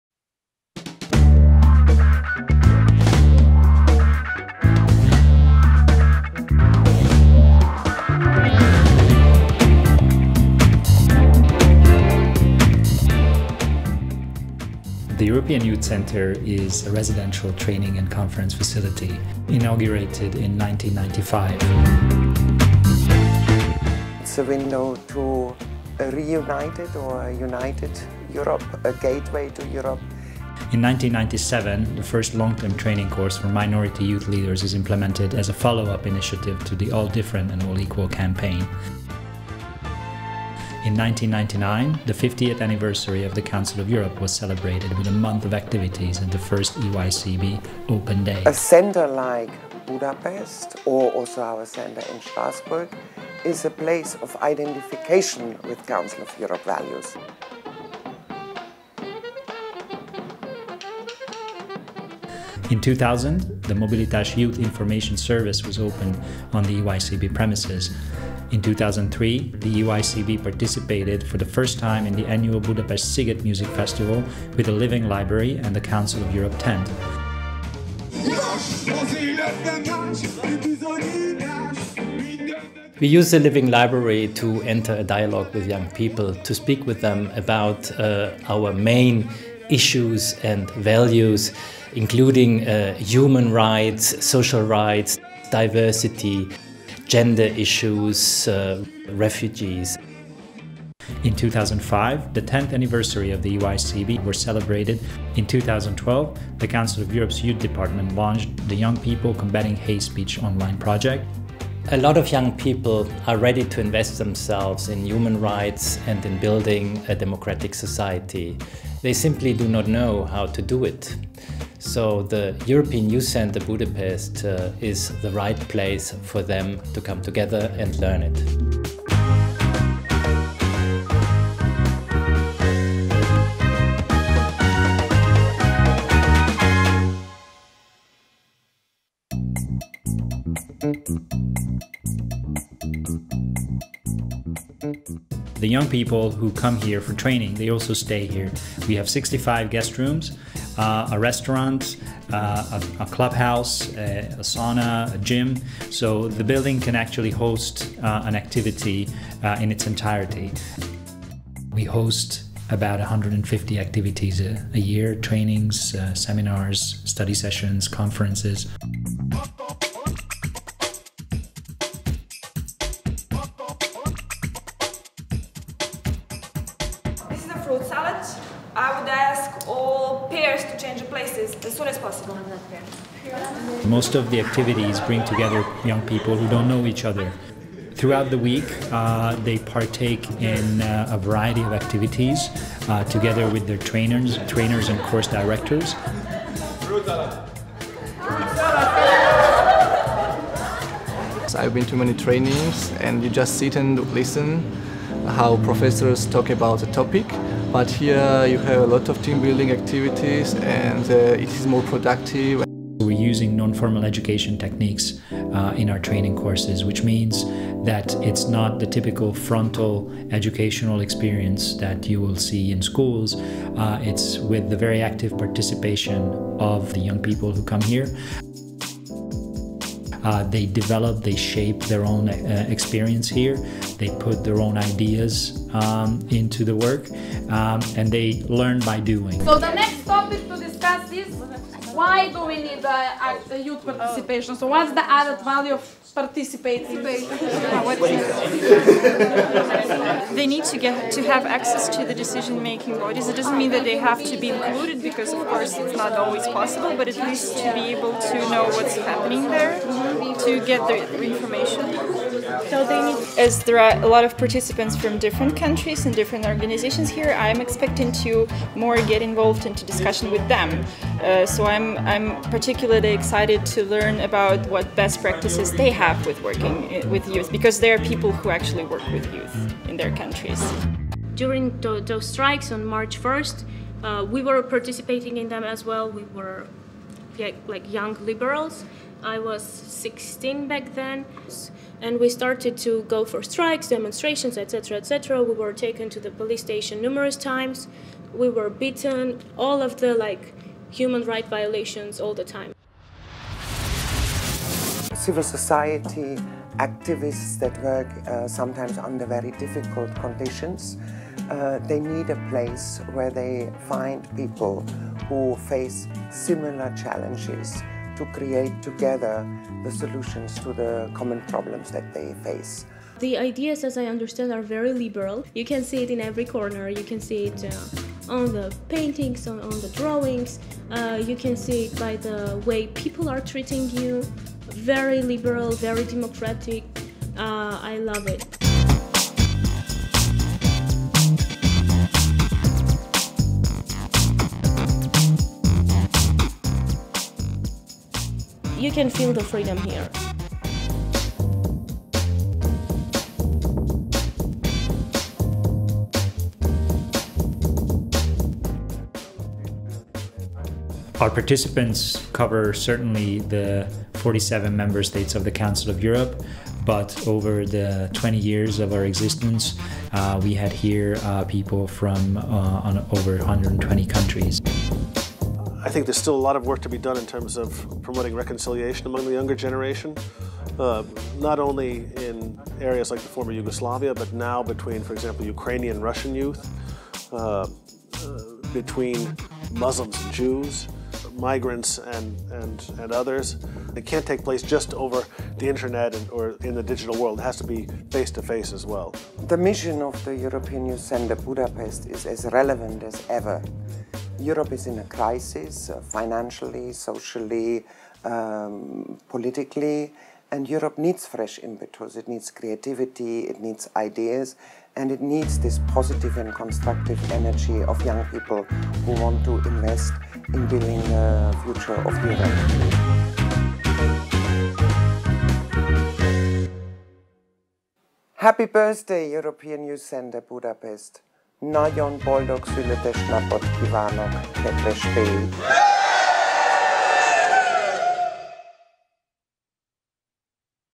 European Youth Center Budapest Image Film
We created this film for the 20th anniversary of EYCB. It was fun shooting 3 days in this wonderful building among intelligent creative young people.